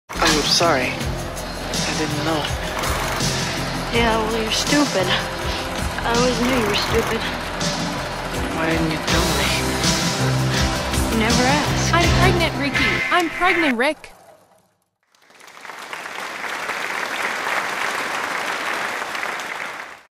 trendingdogvideo comedy comedyshorts shortsviral shorts short shortvideo poodles
Cute way to say we are expecting a litter of Standard Poodles all shades of amber.